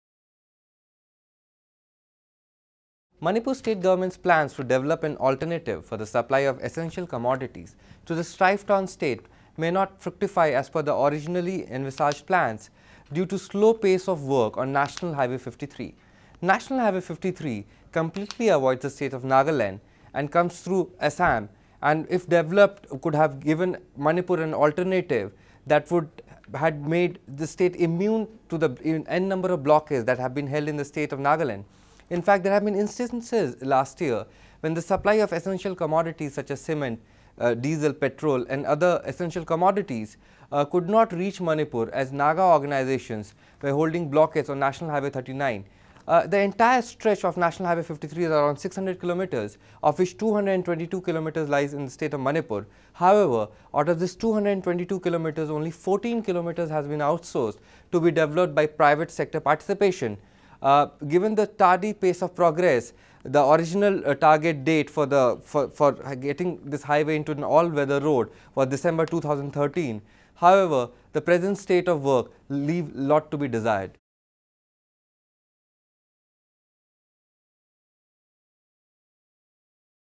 A road going nowhere
Highway blockades have crippled Manipur in the past. But Mint's Utpal Bhaskar says the bid to develop an alternative road through Assam is
unlikely to bear fruit by next year.